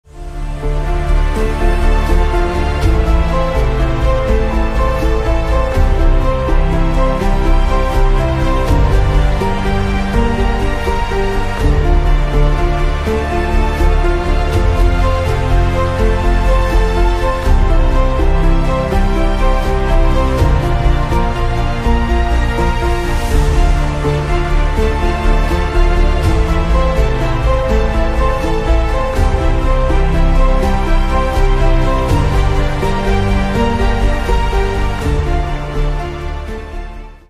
Incredible transformation of Ocean animals into warriors using AI
In an incredible transformation of Killer whale, Starfish and Sea urchin. using artificial intelligence tools (computer learning technology) into warriors from horror movies.